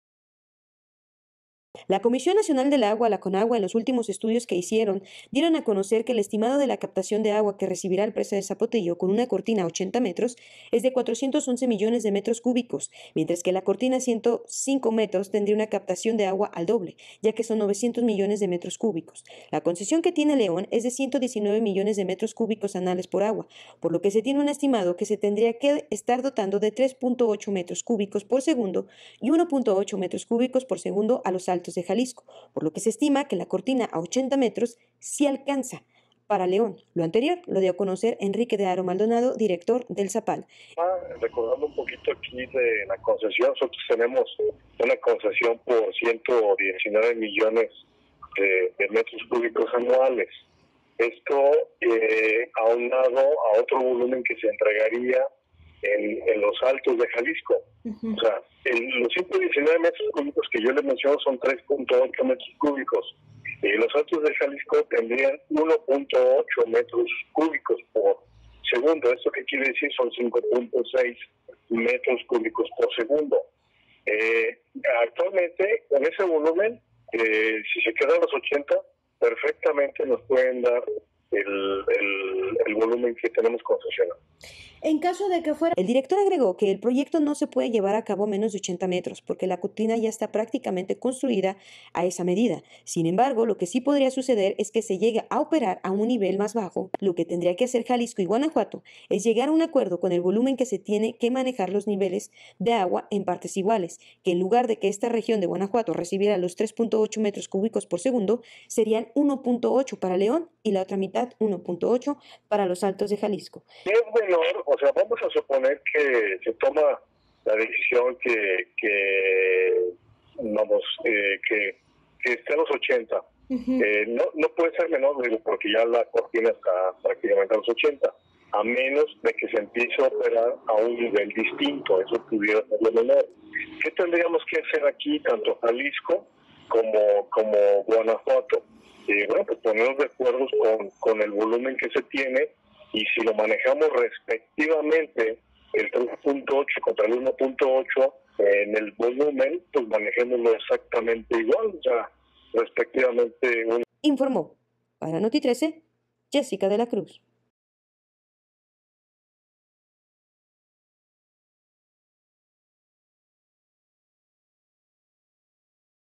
Captación de agua para El Zapotillo si alcanza para cubrir demanda
"Perfectamente se puede cubrir el volumen de agua que se demanda"; Estudios revelaron que la cantidad de agua que captaría la presa de 'El Zapotillo' con una cortina de 80 metros, sería más que suficiente para abastecer la ciudad de León y los Altos de Jalisco

Dalila Gallegos 7:00 a.m a 9:00 a.m

Camila Islas de 13:00 a 15:00 Hrs.

Juan Carlos Espinosa de 21:00 a 22:30 Hrs.